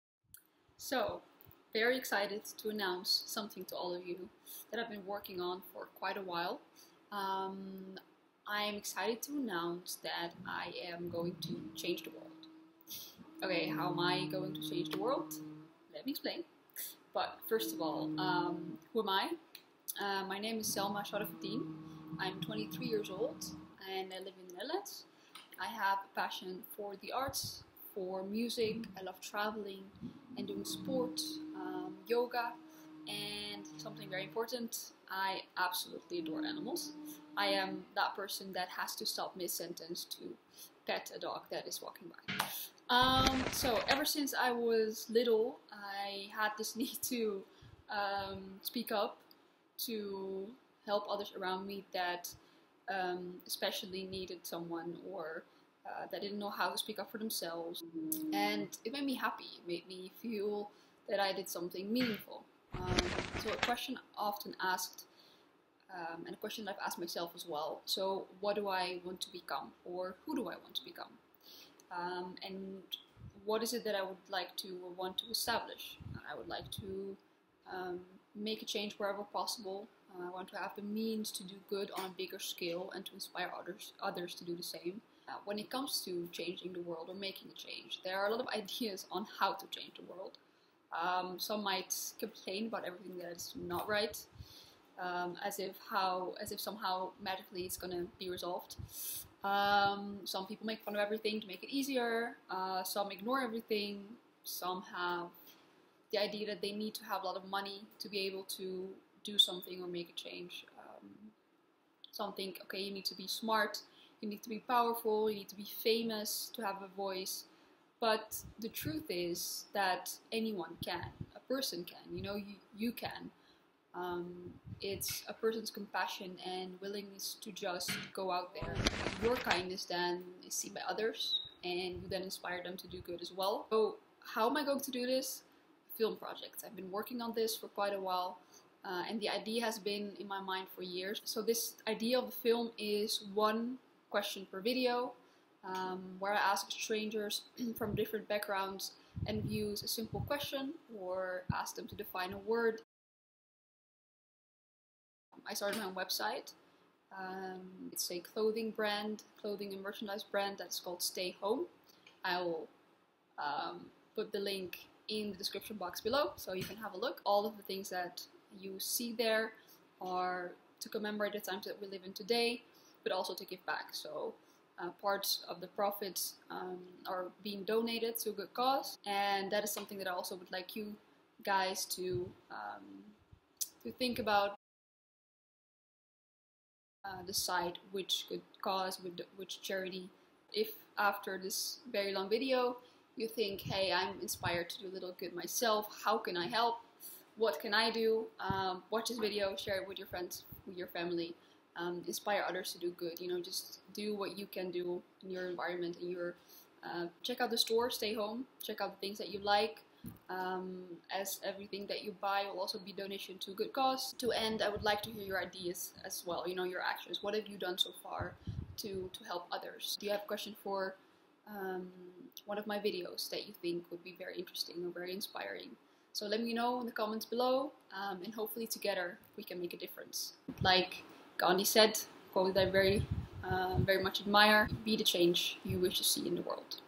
I am going to change the world - Film Project!
Excited to announce, I am going to change the world!

How am I going to CHANGE the world?!

Oke, let me explain…

CLOTHING and MERCH = StayHome = donation to a charity of your choice =
Let me know which charity in the comments below.  ↓↓↓

GEAR USED:
My camera

Hmmmkay  Bye

xoxo Selma

Let me ALSO know what your thoughts are on this video and/or if you have a great idea for a next question?!